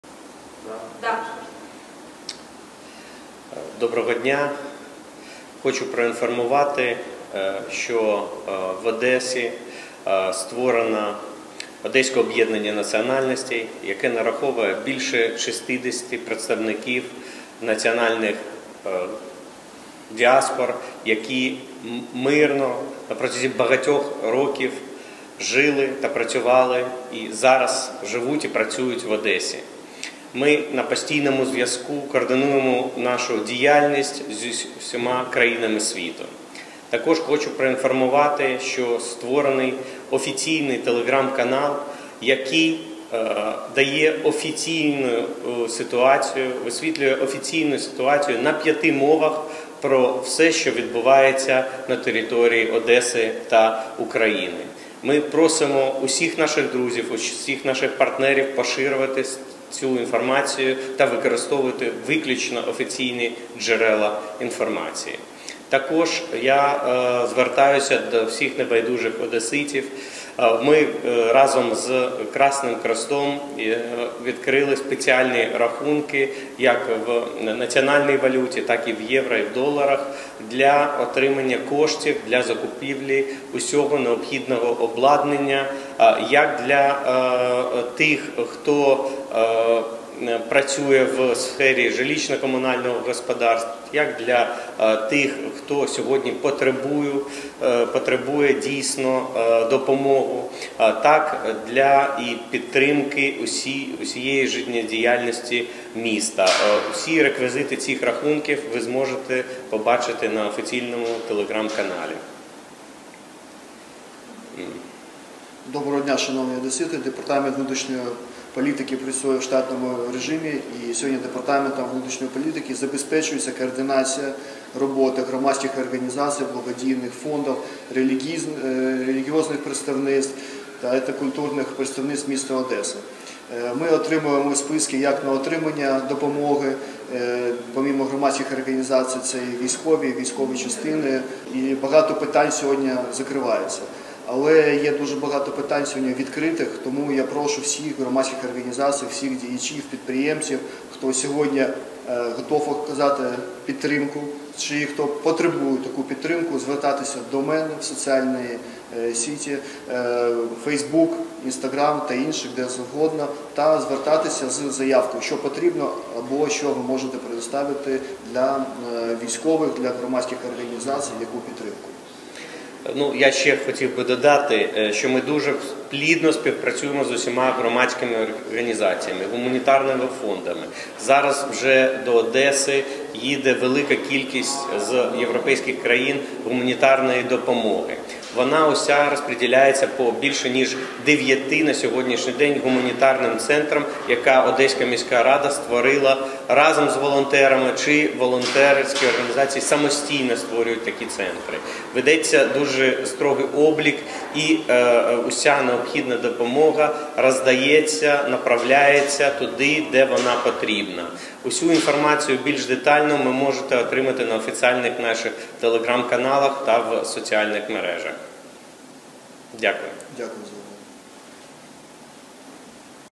Одеса на зв'язку з усіма країнами світу
Віце-мер Павло Вугельман: В Одесі створено і працює одеське об’єднання національностей, яке сьогодні плідно співпрацює з міськрадою та волонтерами, збираючи допомогу населенню та армії. Також важливим є те, що представники діаспор доносять позицію нашої країни, нашого міста своїм друзям та родичам по всьому світу. Нагадаю, що Червоним Хрестом в Одеській області створено спеціальний рахунок, на який можна надіслати кошти для допомоги містянам. Реквізити можна отримати в офіційних телеграм-каналах міста та на офіційному сайті м. Одеса.

Також, за словами директора департаменту внутрішньої політики Олександра Жильцова, мерія активно співпрацює з громадськими, волонтерськими та релігійними організаціями міста, які надають допомогу одеситам. Усі, хто бажає приєднатись можуть звертатись до департаменту.